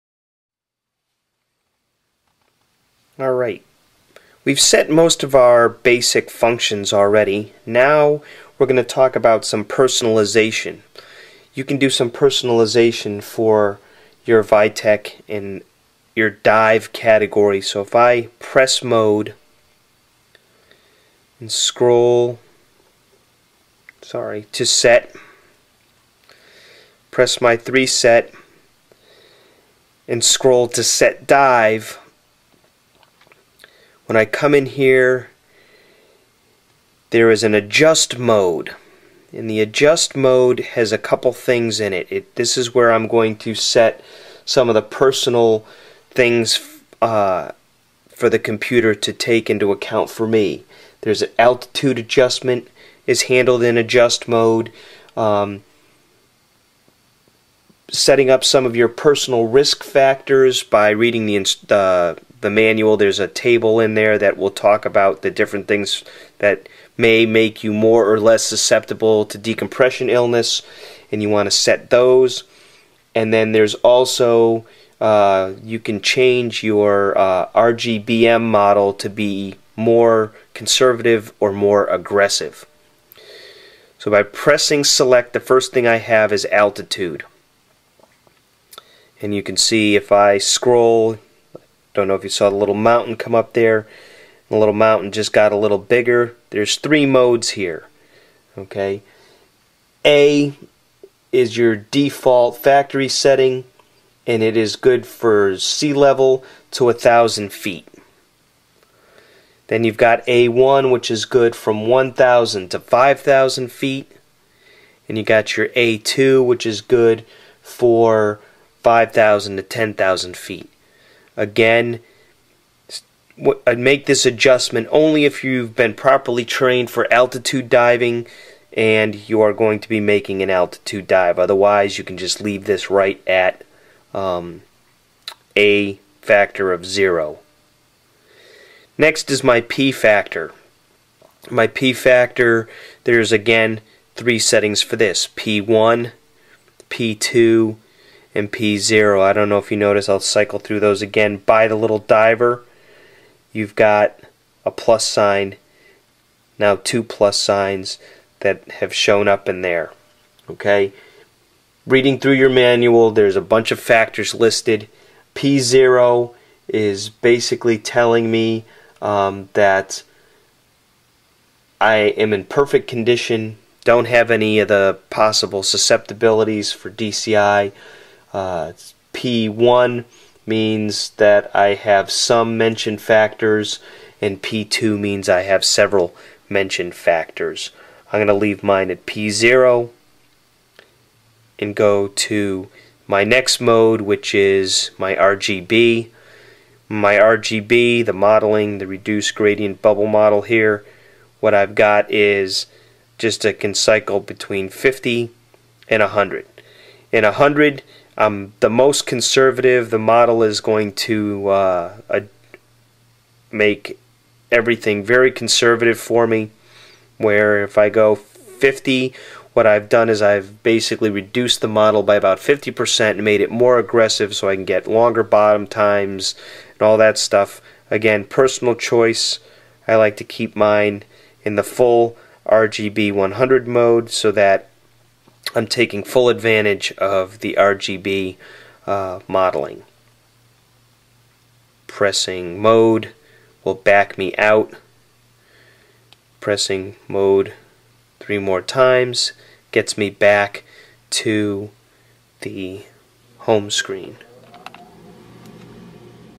Suunto Vytec non DS 10 setting altitude, personal settings and RBG model
Setting the computer for altitude diving, personal factors that make you more or less susceptible to DCS and how aggressive or conservative the RBG model is on the Suunto Vytec non DS model dive computer